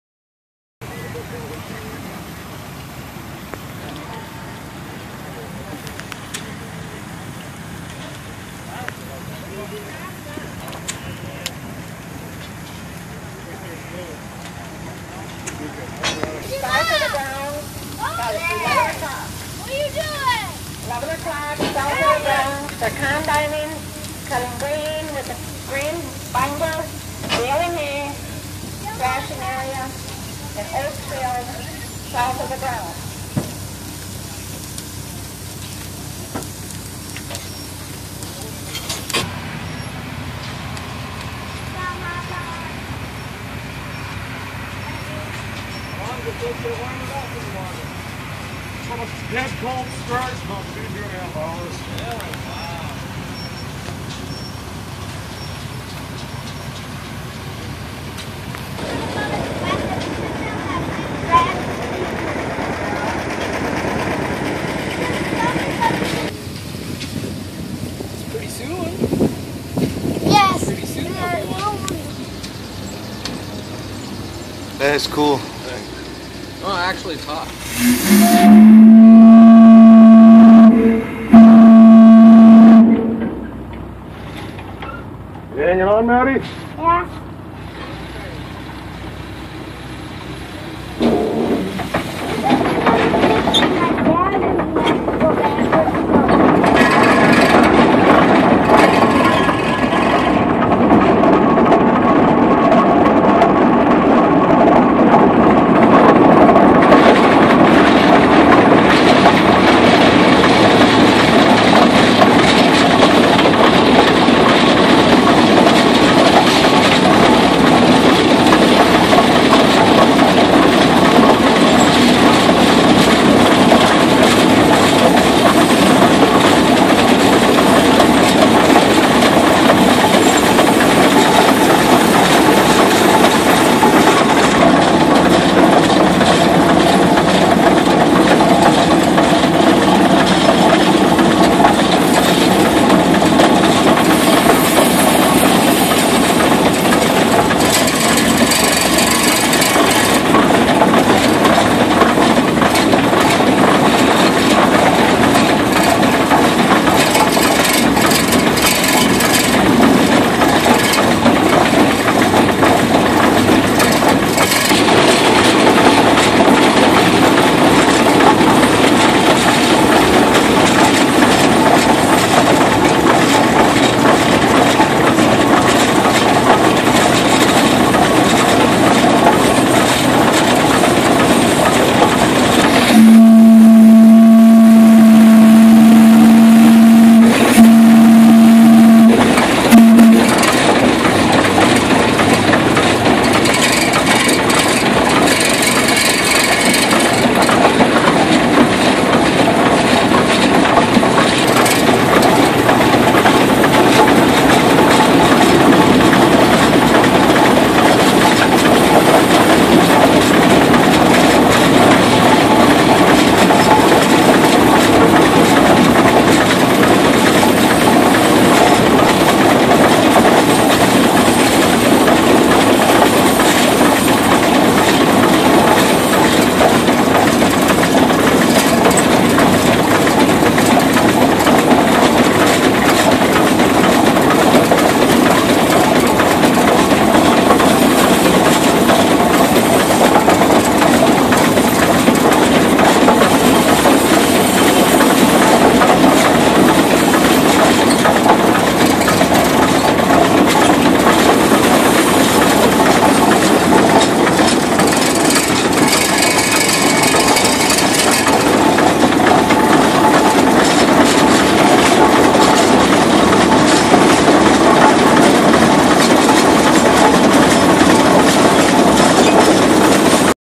Riding a Rumely Steam tractor!
The engineer offered me a ride in this massive tractor at the 2014 Badger Steam and Gas Show in Baraboo, Wi.